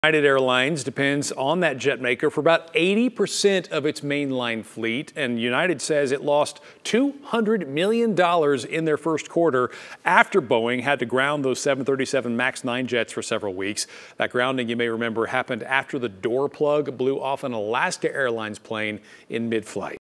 United Airline reports $200 million loss in first quarter due to Boeing troubles
United heavily depends on Boeing for its mainline fleet.